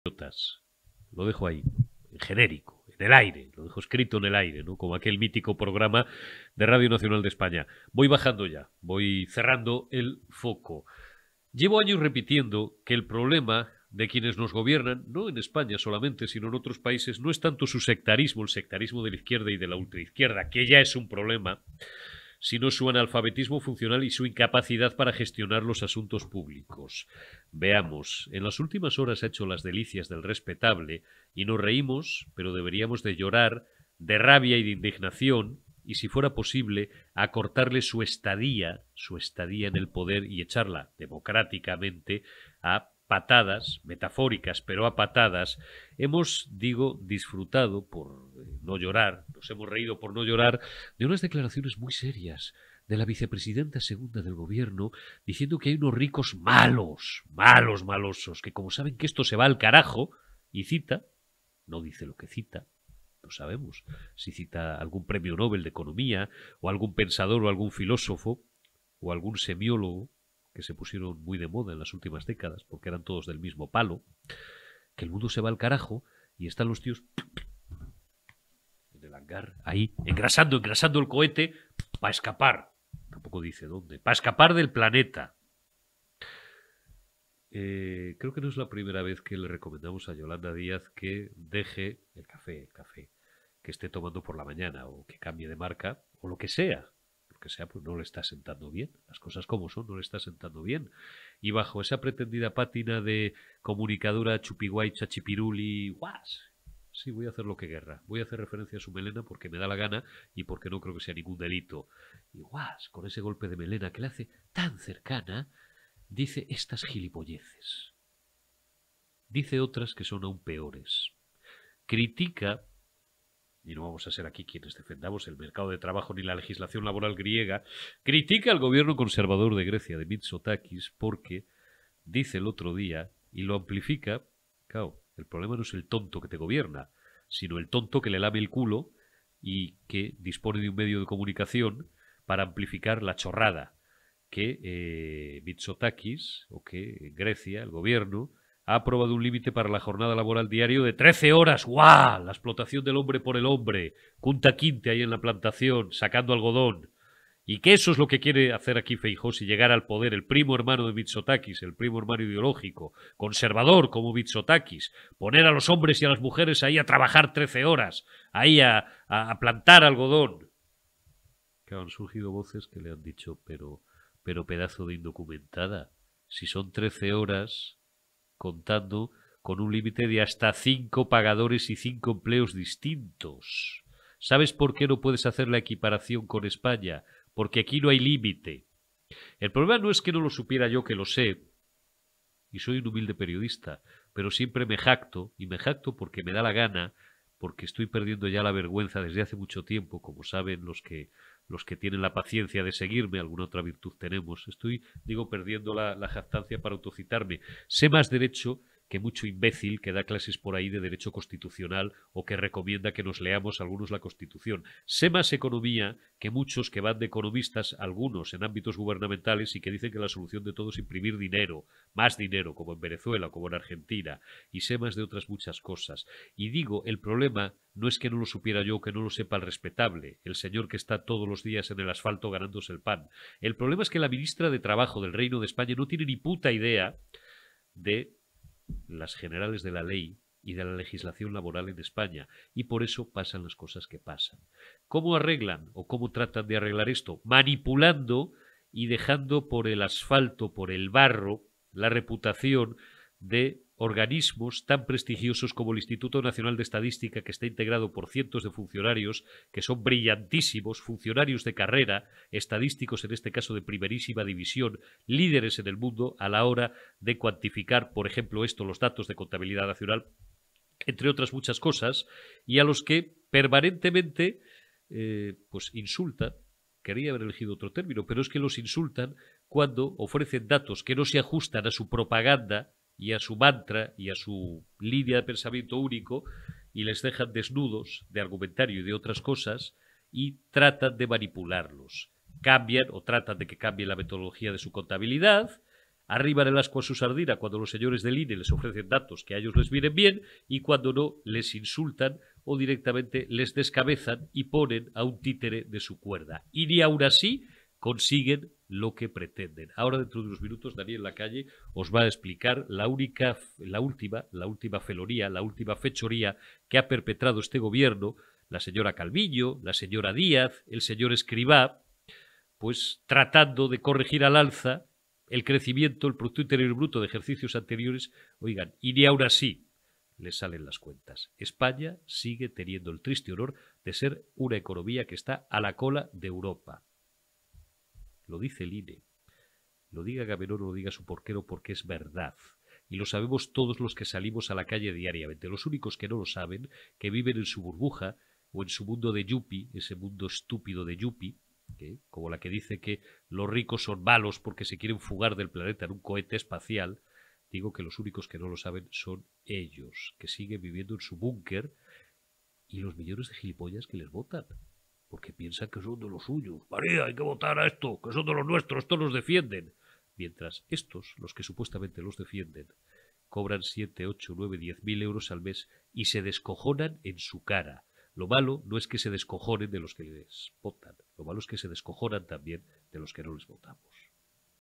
Eurico Campano: “No estamos en manos de sectarios, estamos en manos de analfabetos”
Eurico Campano analiza en su editorial las declaraciones de Yolanda Díaz en un mitin de este fin de semana, donde se preocupa por las “fugas en naves espaciales y cohetes” de las grandes clases. La conclusión del presentador del programa es que estamos en manos de analfabetos; una líder capaz de preocuparse por “los que huyen del mundo”, es la misma persona que negocia con Puigdemont en Bélgica.